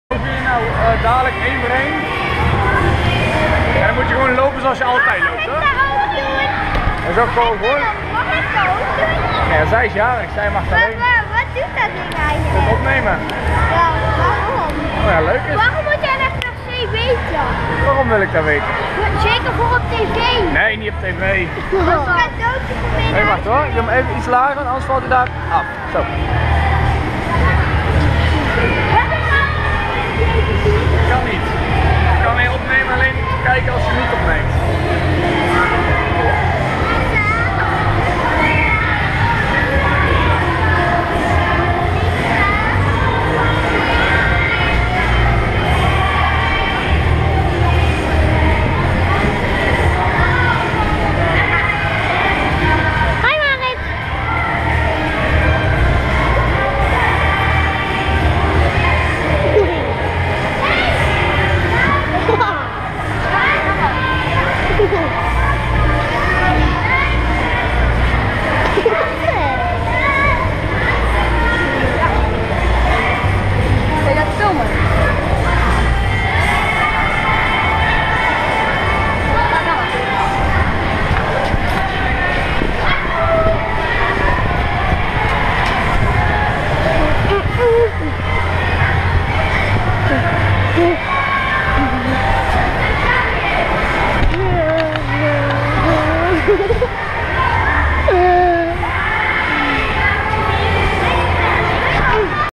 Door de ogen van een kind - Zevensprong 2013
Een kind tijdens het zeskamp een GoPro op het hoofd zetten en laten spelen.

Over een tijdje komt er een mooie compilatie van de hele week Zevensprong!